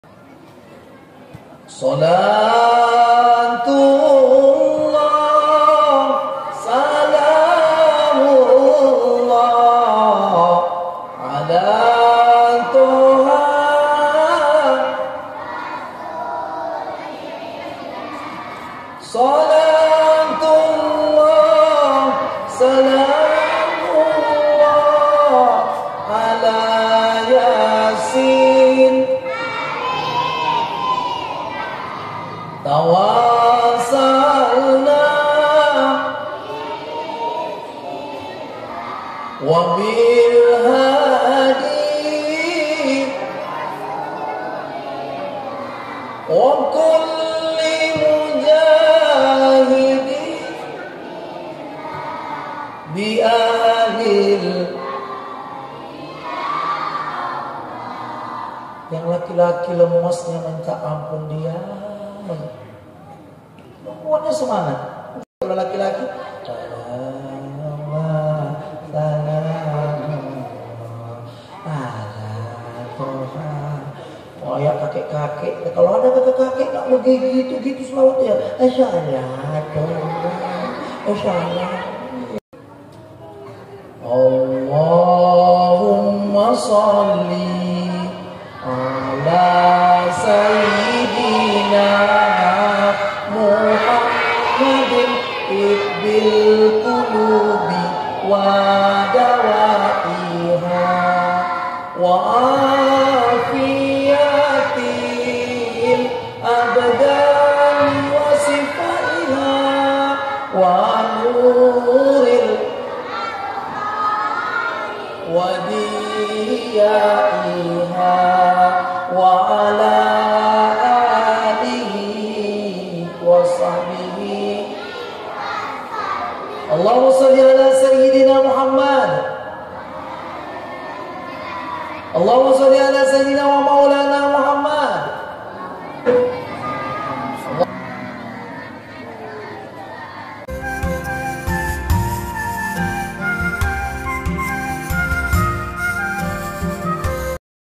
MENANTI BUKBER DI AL-MUKHLISIN || NAK MARI BERSHOLAWAT
acara bukber disebuah sekolah,TK,MDTA,MIS Almukhlisin disebuah desa Kuala Tanjung dibawah binaan yayasan Saudara Untung.Acara ini merupakan rutinitas tiap tahun dilakukan disekolah ini dalam rangka membina generasi Islam yg Sholeh dan sholeha.

 Dakwah kajian
My Tube Studio Video Channel
Mohasafherman Adbarah Video Channel
MAVC